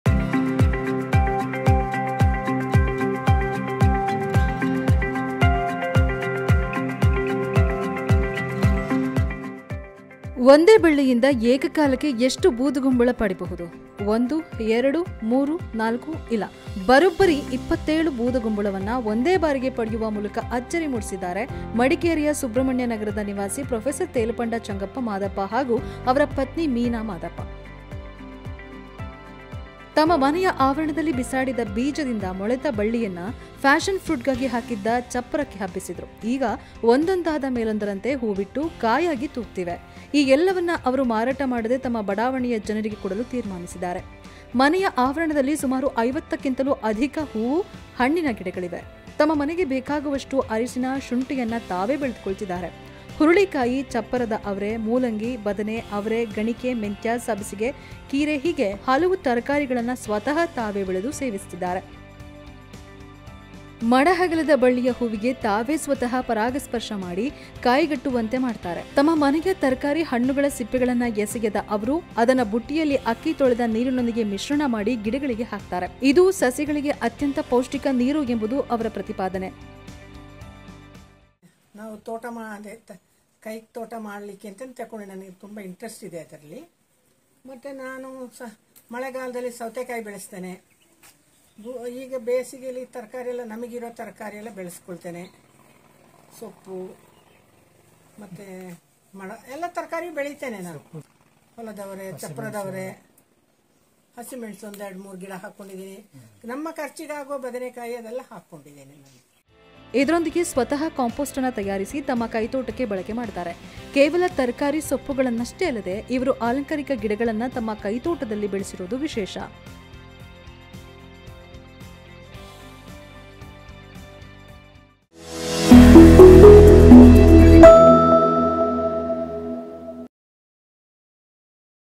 ಒಂದೇ ಬಳ್ಳಿಯ ಬೂದುಗುಂಬಳಗಳು.. ‌ | Boodugumbala | Madikere | Ash Gourd
ಬರೋಬ್ಬರಿ 27 ಬೂದುಗುಂಬಳವನ್ನು ಒಂದೇ ಬಾರಿಗೆ ಪಡೆಯುವ ಮೂಲಕ ಅಚ್ಚರಿ ಮೂಡಿಸಿದ್ದಾರೆ ಮಡಿಕೇರಿಯ ಸುಬ್ರಹ್ಮಣ್ಯನಗರದ ನಿವಾಸಿ ಪ್ರೊ.ತೇಲಪಂಡ ಚಂಗಪ್ಪ ಮಾದಪ್ಪ ಹಾಗೂ ಅವರ ಪತ್ನಿ ಮೀನಾ ಮಾದಪ್ಪ.ತಮ್ಮ ಮನೆಯ ಆವರಣದಲ್ಲಿ ಬಿಸಾಡಿದ ಬೀಜದಿಂದ ಮೊಳೆತ ಬಳ್ಳಿಯನ್ನು ಫ್ಯಾಷನ್‌ ಫ್ರೂಟ್‌ಗಾಗಿ ಹಾಕಿದ್ದ ಚಪ್ಪರಕ್ಕೆ ಹಬ್ಬಿಸಿದರು. ಈಗ ಒಂದಾದ ಮೇಲೊಂದರಂತೆ ಹೂ ಬಿಟ್ಟು, ಕಾಯಾಗಿ ತೂಗುತ್ತಿವೆ.